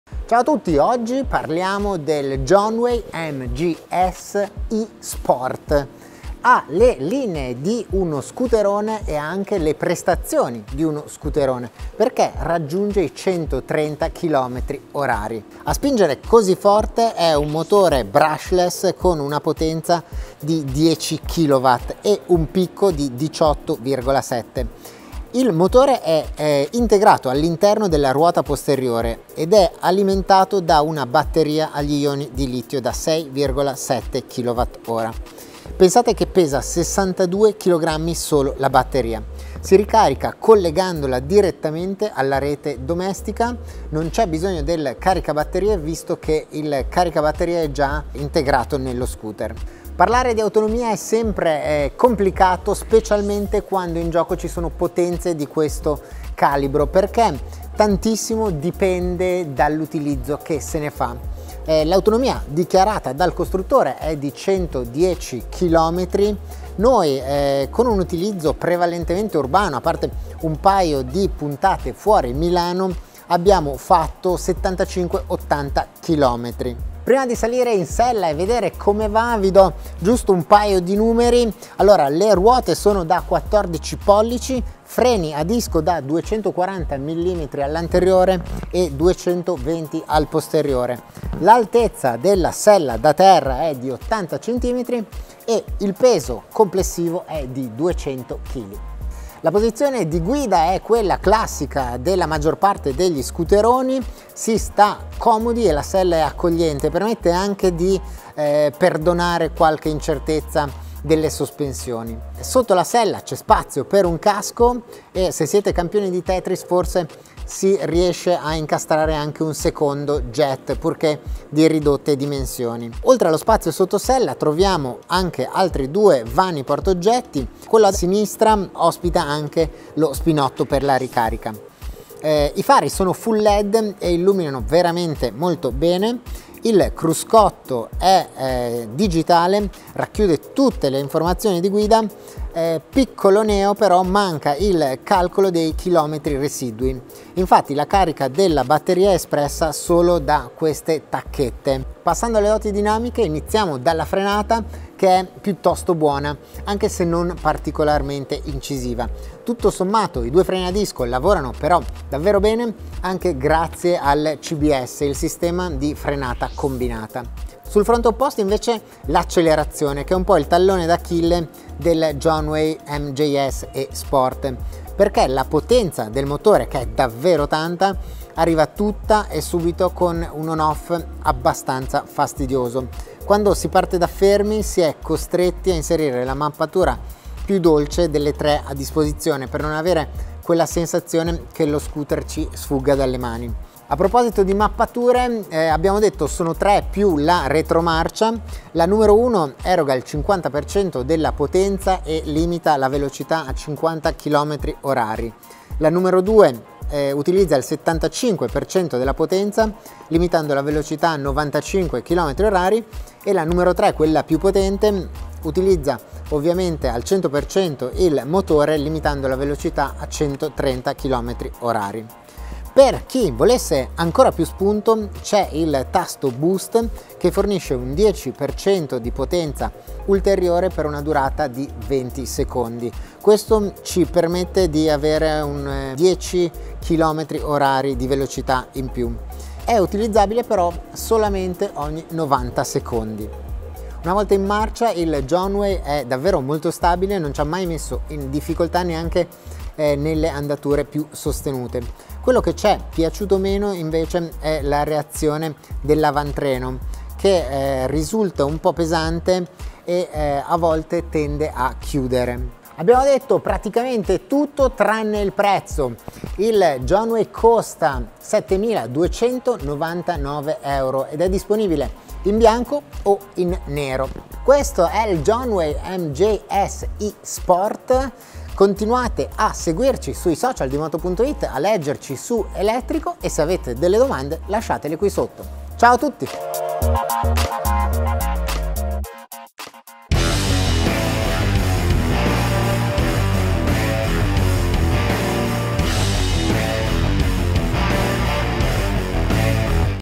TEST - Jonway MJS-E sport. Elettrico da 130 km/h
Tantissima potenza, anche troppa e non sempre facilmente gestibile. L'elettrico diventa adulto con uno scooter made in Cina molto veloce e delle linee piacevoli